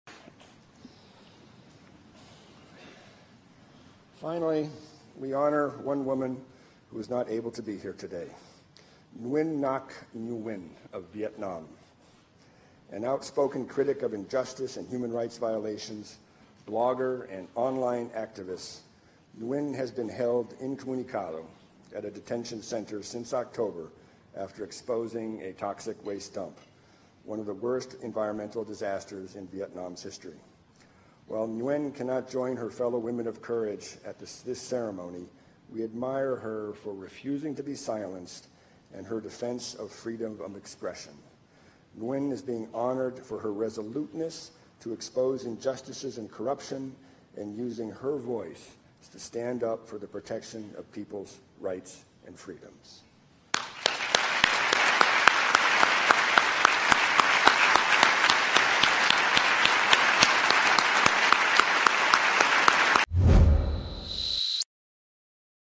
Blogger Mẹ Nấm nhận giải thưởng của Bộ Ngoại giao Hoa Kỳ
Cuối cùng, chúng tôi muốn vinh danh một phụ nữ, người đã không thể có mặt ở đây ngày hôm nay, bà Nguyễn Ngọc Như Quỳnh đến từ Việt Nam. Bà là một tiếng nói mạnh mẽ chống lại những sự bất công, vi phạm nhân quyền, một blogger và nhà hoạt động trên mạng. Bà Quỳnh đang bị giam giữ, không cho liên lạc với bên ngoài kể từ tháng 10 năm ngoái vì đã vạch trần một vụ xả chất độc ra môi trường, một t rong những thảm hoạ môi trường tồi tệ nhất trong lịch sử Việt Nam.
Ngày hôm nay, bà Quỳnh không thể có mặt bên cạnh những phụ nữ can đảm khác trong buổi lễ này. Chúng tôi ngưỡng mộ bà vì đã không im lặng, vì đã đứng lên bảo vệ quyền tự do biểu đạt.
Chúng tôi vinh danh Nguyễn Ngọc Như Quỳnh vì đã quyết tâm vạch trần tham nhũng, bất công, và dám đứng lên góp tiếng nói của mình để bảo vệ quyền và tự do của người dân.